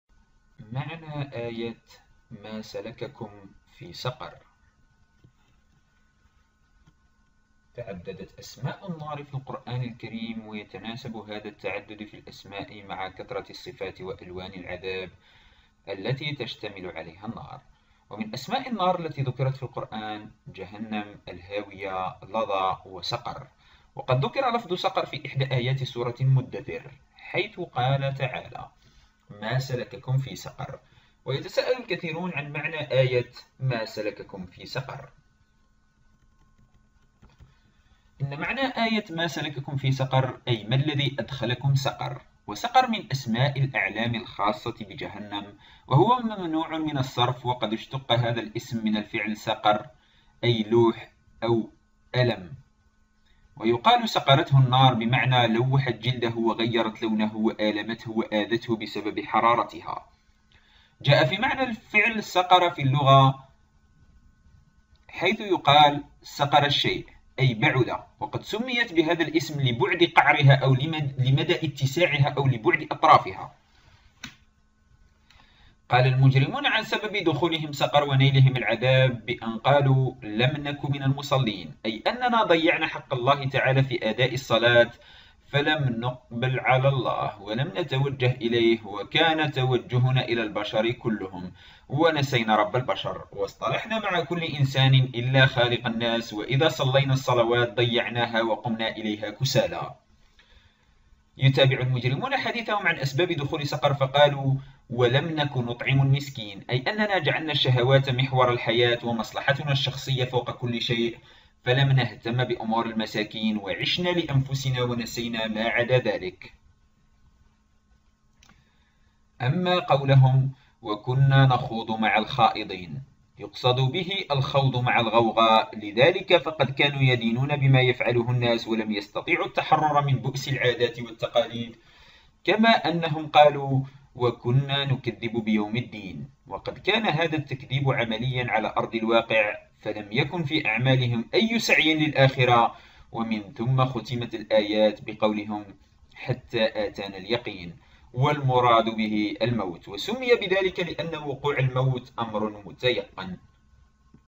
من المقصود في الآية : مَا سَلَكَكُمْ فِي سَقَرَ ؟
تعددت أسماء النار في القرآن الكريم، ويتناسب هذا التعدد في الأسماء مع كثرة الصفات، وألوان العذاب الذي تشتمل عليها النار، ومن أسماء النار التي ذُكرت في القرآن: جهنم، والهاوية، ولظى، وسقر، وقد ذُكر لفظ سقر في إحدى آيات سورة المدثر حيث قال تعالى: (ما سلككم في سقر)، ويتساءل الكثيرون عن معنى آية ما سلككم في سقر.
There are many names of fire in the Holy Qur’an, and this multiplicity of names corresponds to the many attributes and the colors of torment that fire contains. Among the names of fire that were mentioned in the Qur’an are: Hell, the Abyss, Laza, and Saqr. The word Saqr was mentioned in one of the verses of Surat Al-Muddathir, where he said Almighty: (He did not walk you in Saqr), and many wonder about the meaning of the verse that led you in Saqr.